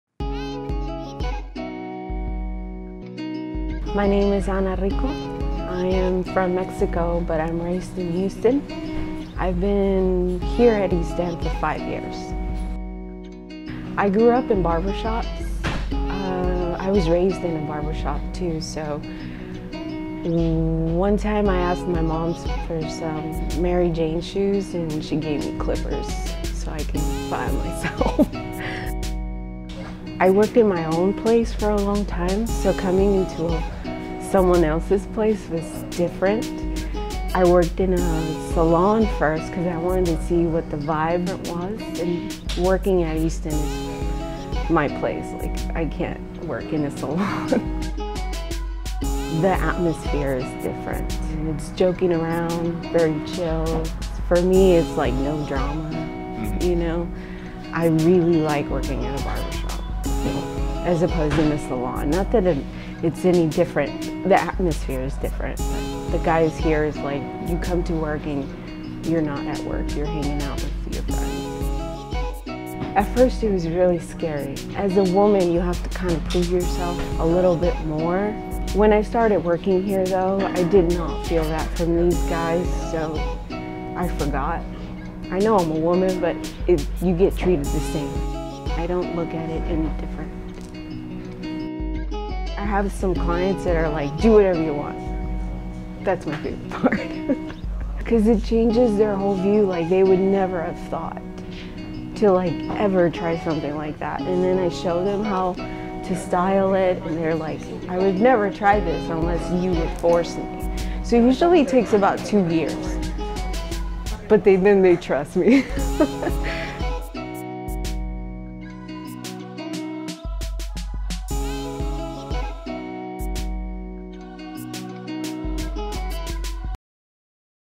The Shop Backbone - Ana Rico
Miss Ana takes us through her journey as a barber from growing up in barber shops to creating her own style in the craft.